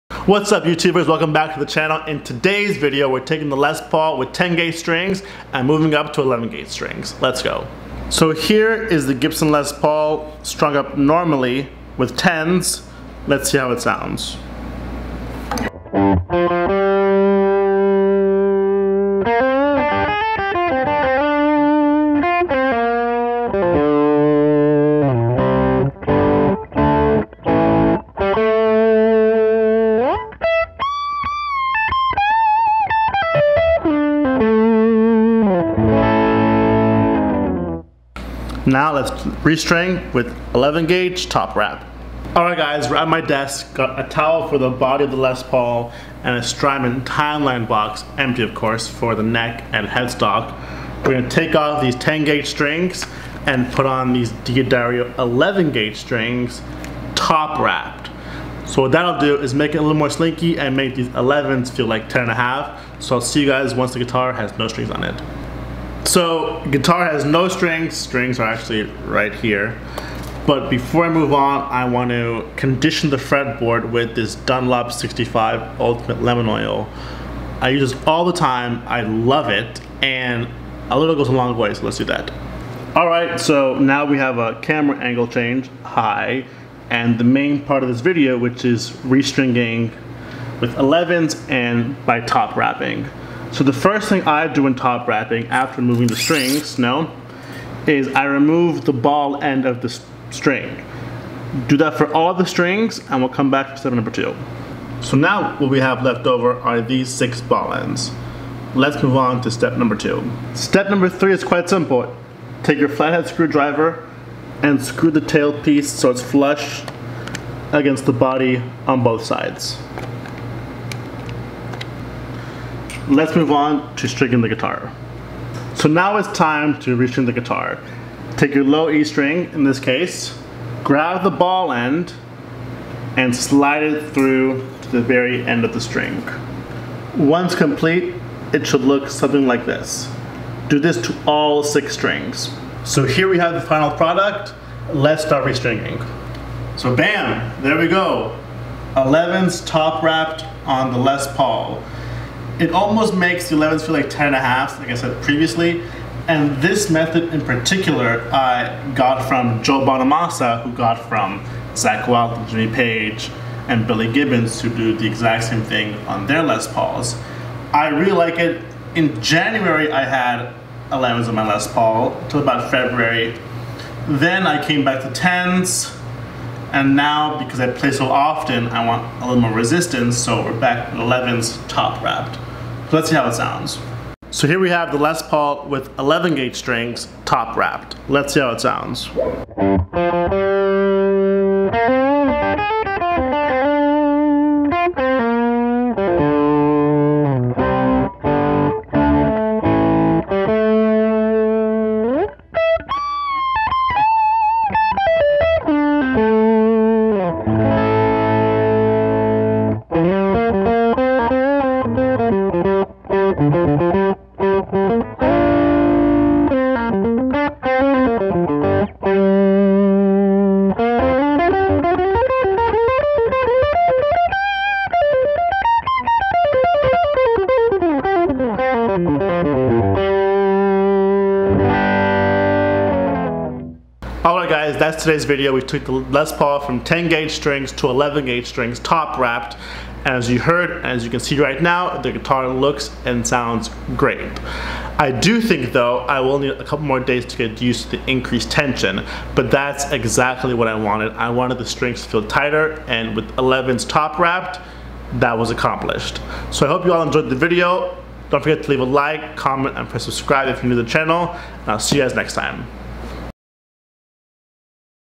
Going from 10s to 11s!
Whats up guys, welcome back to the channel. In todays video I'm changing my strings from 10s to 11s, and top wrapping them. 10s are becoming too easy for me so going to the 11s will add a bit more tension, which is what I want.